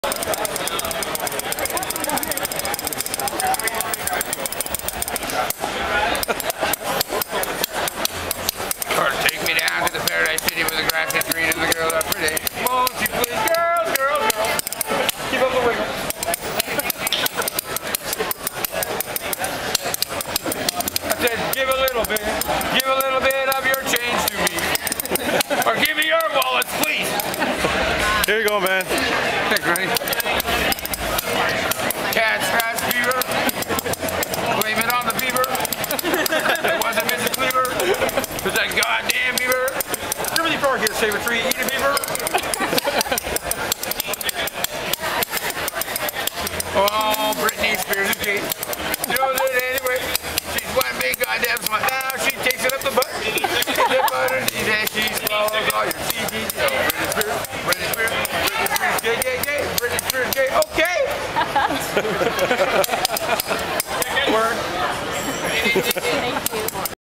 Spoonman
Awesome bum in front of The Commodore in Vancouver before the Carcass show